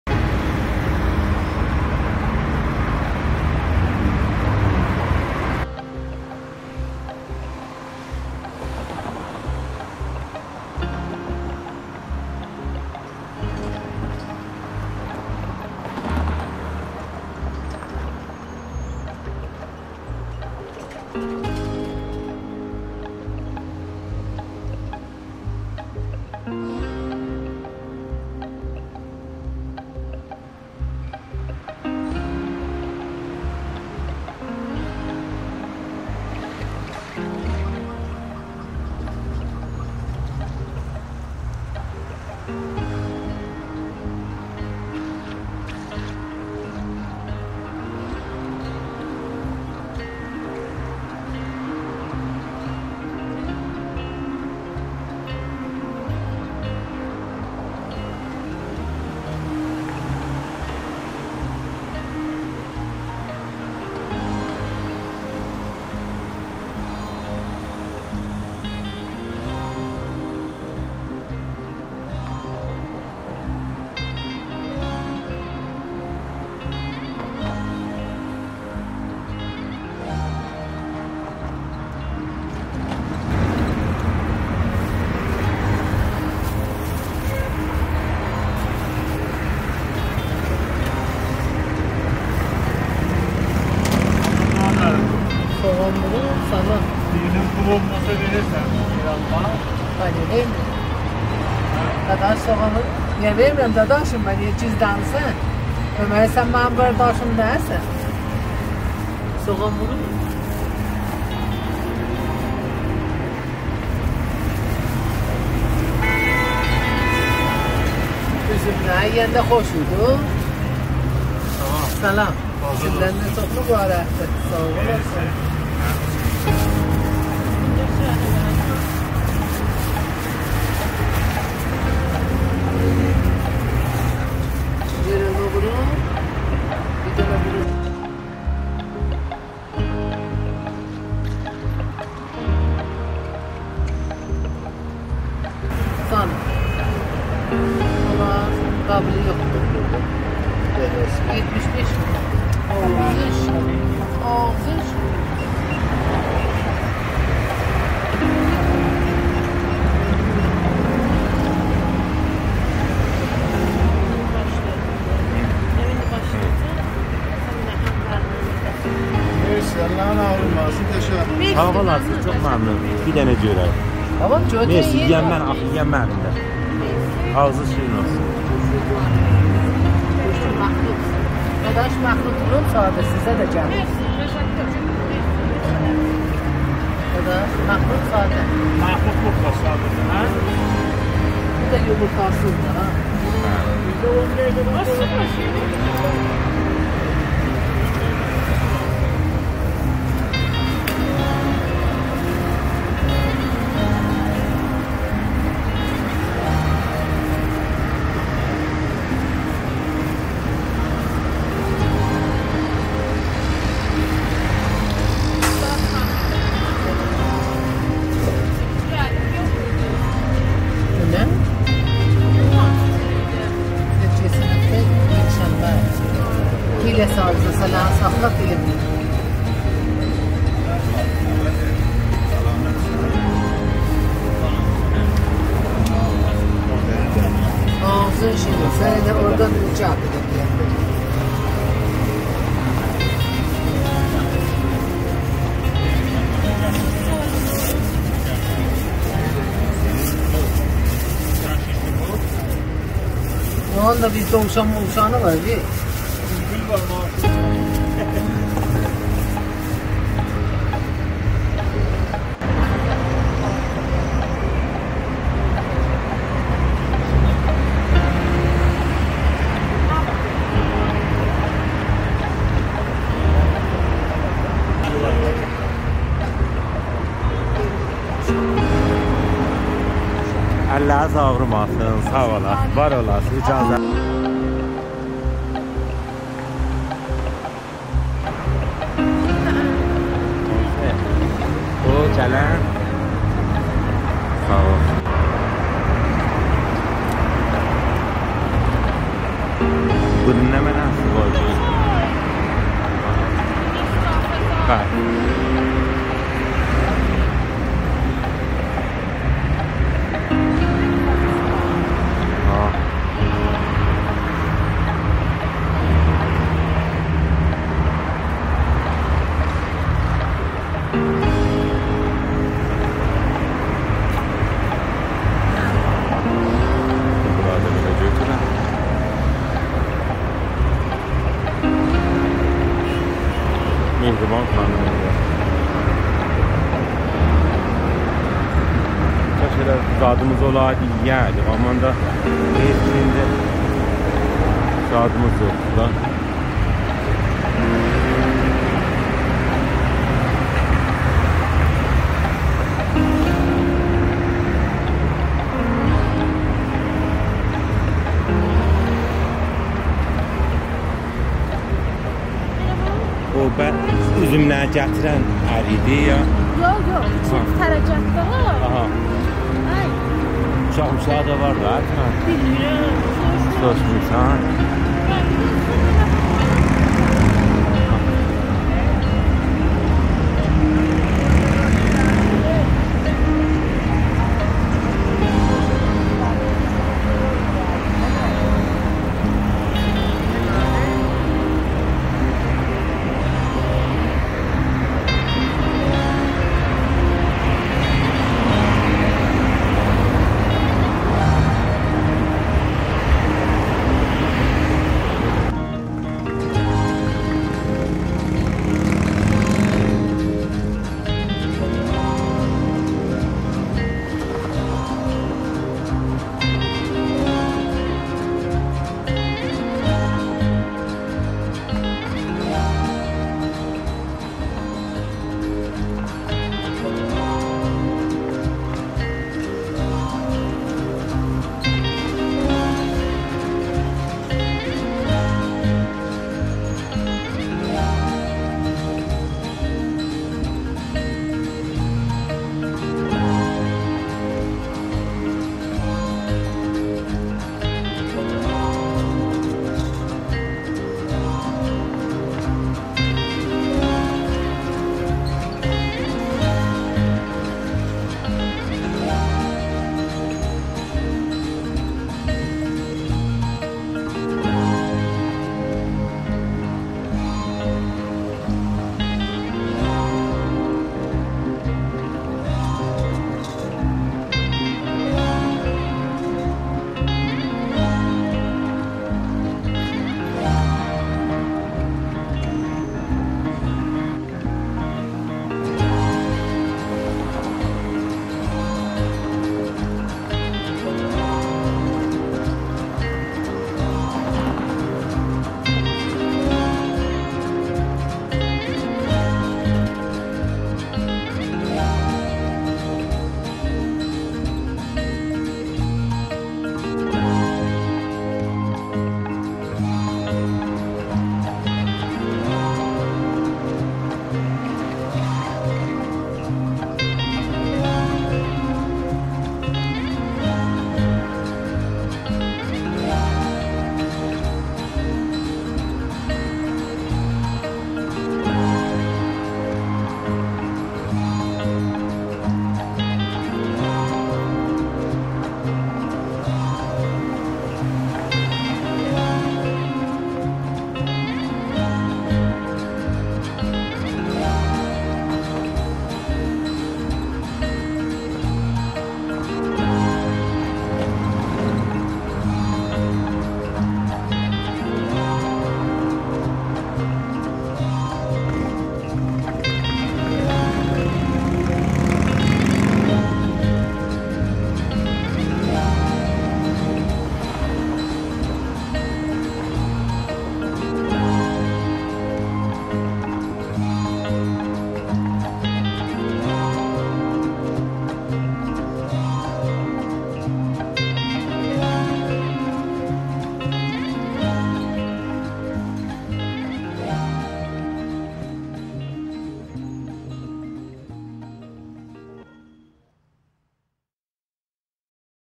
Orumia Street Foods | Night Life | Traditional Foods Taste | Orumia - Iran
In this video, we're bringing you a taste of Orumia Street Foods, Oromia's famous nightlife spot, and then continuing on to explore traditional Iranian foods. With over 20 different types of food, you're sure to find something you love!

If you're looking for a night out in Orumia, then this is the video for you! We'll take you on a tour of Orumia Street Foods, and then show you some of the best traditional Iranian foods around. From delicacies like Yer Alma Kababi to soups and sweets, there's sure to be something for everyone in this video!